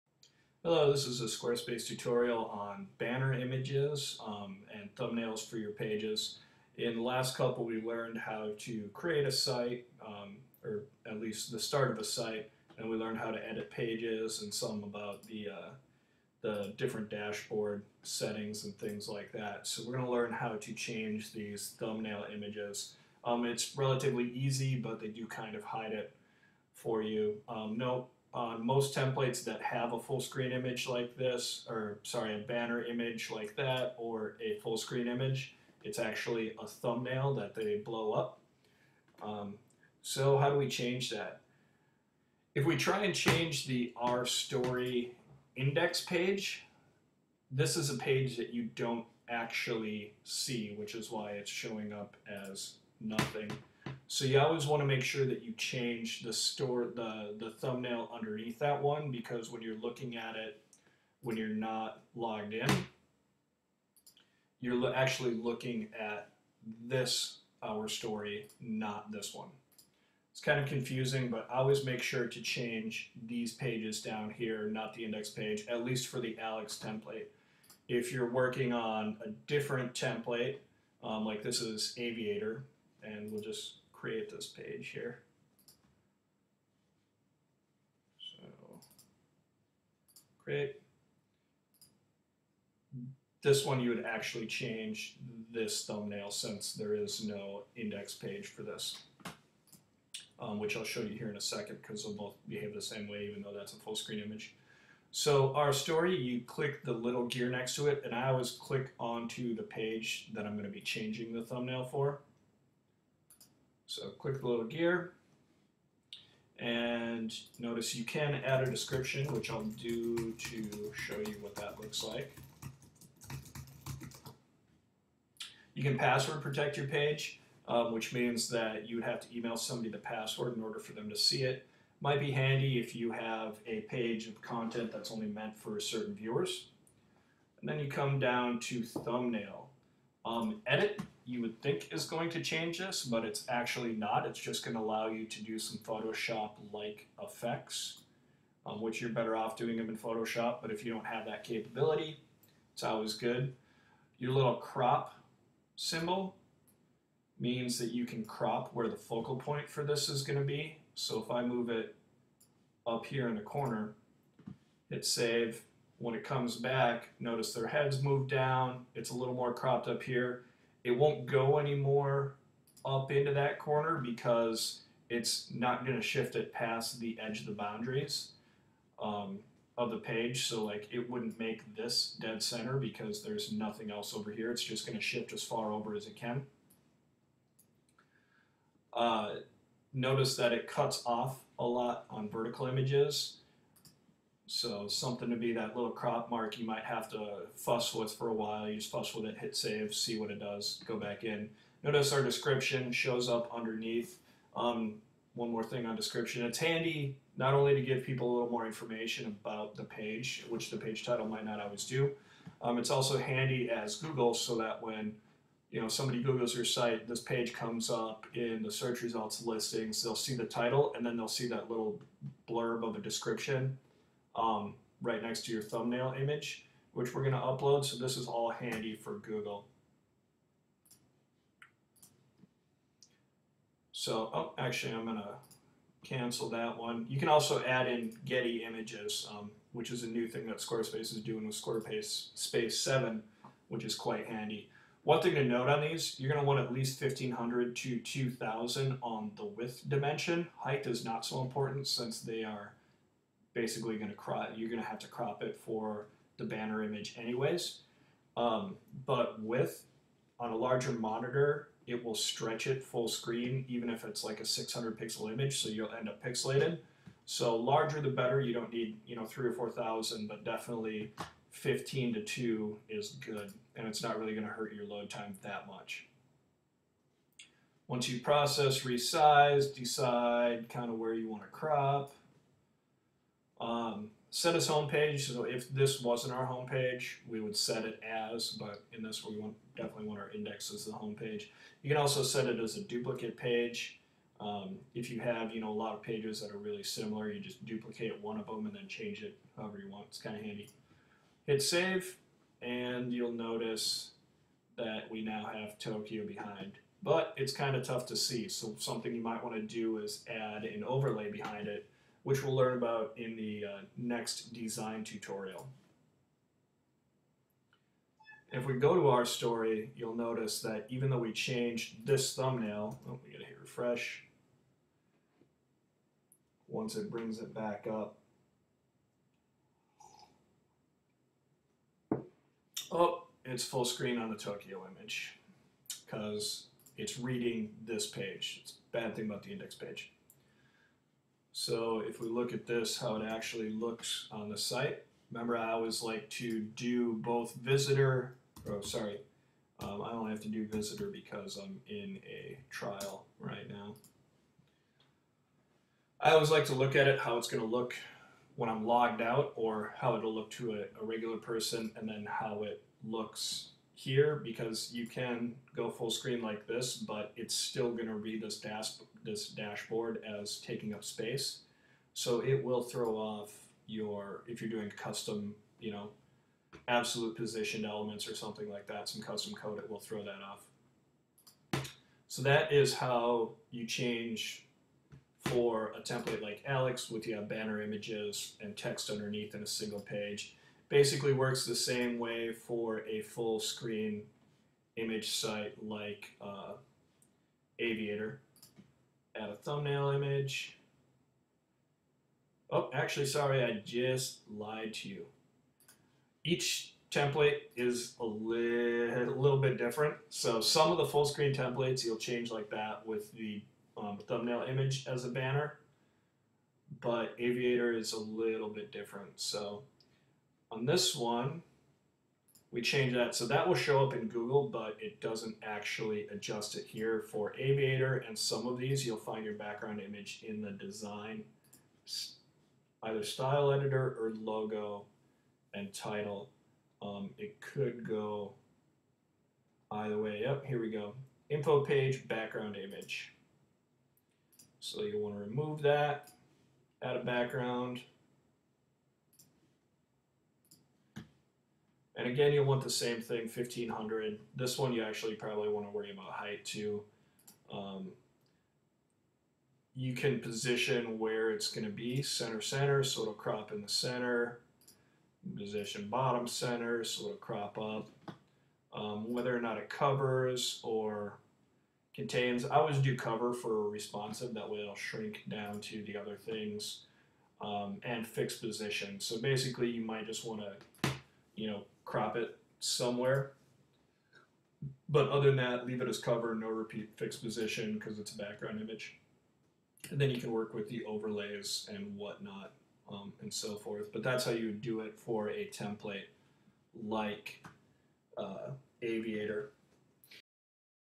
Squarespace Banner & Background Images
Tutorial on setting the banner and background images for Squarespace templates like Alex and Aviator